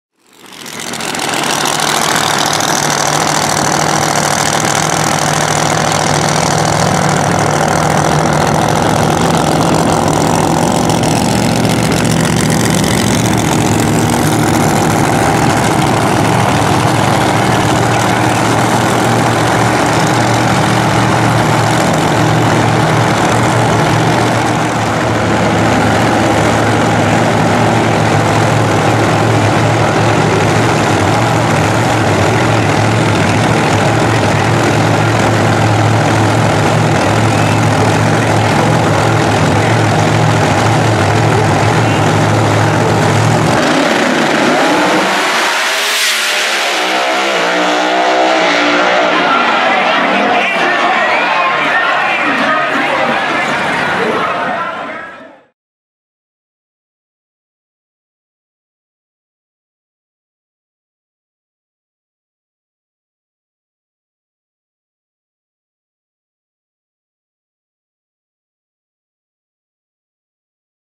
Steve King Superchargers Showdown Maryland International Raceway 10-5-19
STEVE KING MIR 10-5-19 SUPERCHARGERS SHOWDOWN Testing out the savage surface out near the wall!